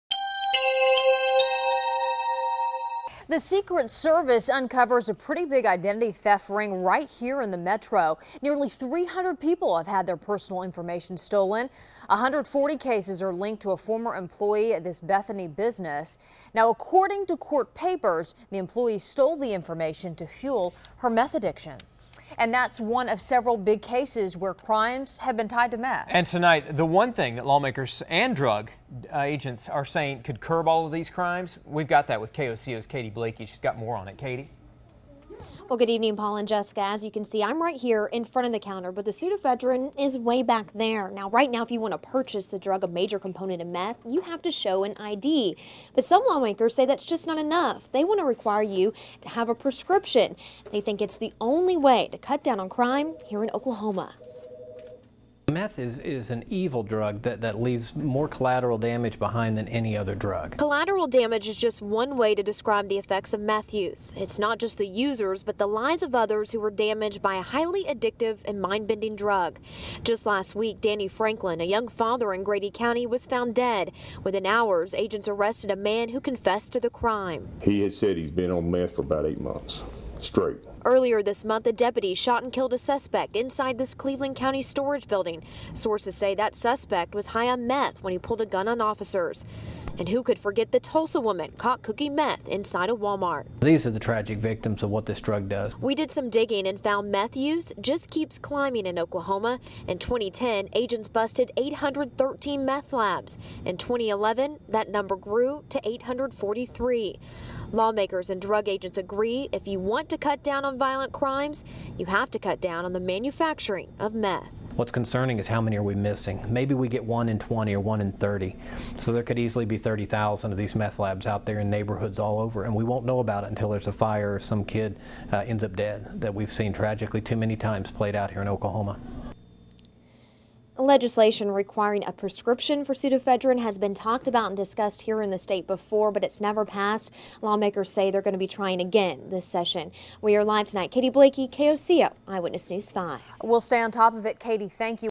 Meth Use, Crimes Continues To Rise
Lawmakers and drug agents are saying there is one thing that can curb these crimes.